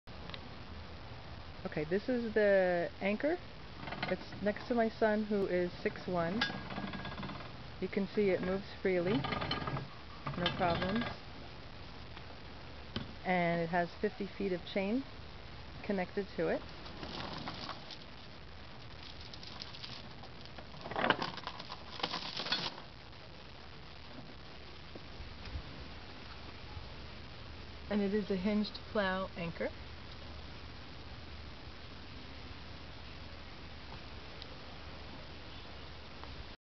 50 lb hinged plow anchor w/ 50 ft of chain
50 lb hinged plow boat anchor, plus 50 ft of chain for sale. Photos attached. $350 takes them both!

A = 33 inches
B = 23 inches